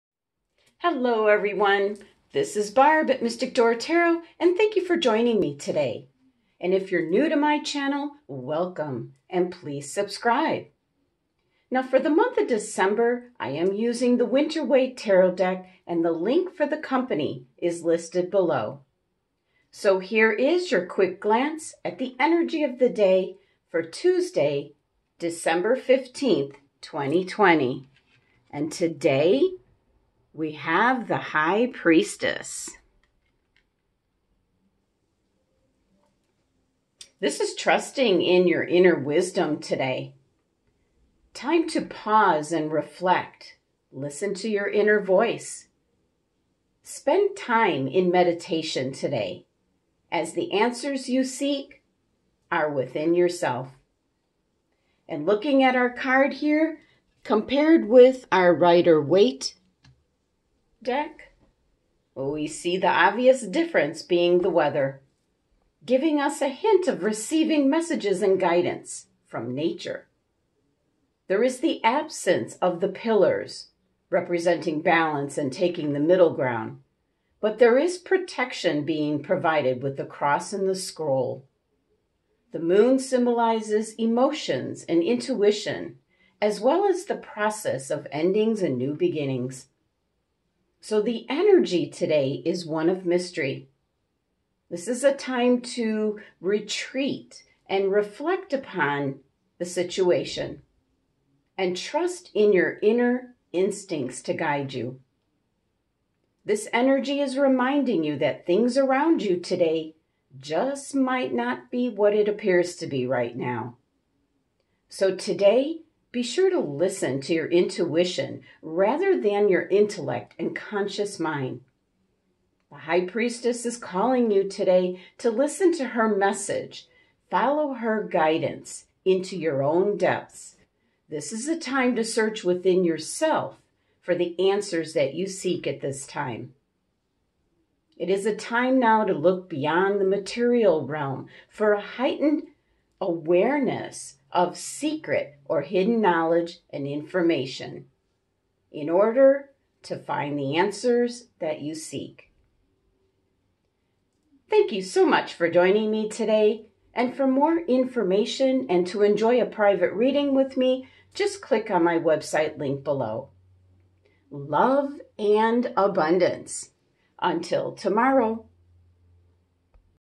Daily Tarot Reading ~ Dec 15, 2020 ~ Mystic Door Tarot ~ DAILY QUICK GLANCE!
Daily Tarot Reading for Dec 15, 2020 by Mystic Door Tarot
Offering general daily tarot, weekly tarot, and monthly tarot readings
TODAY ~ THE (MA) THE HIGH PRIESTESS
This is your quick glance reading for the energy of the day ahead!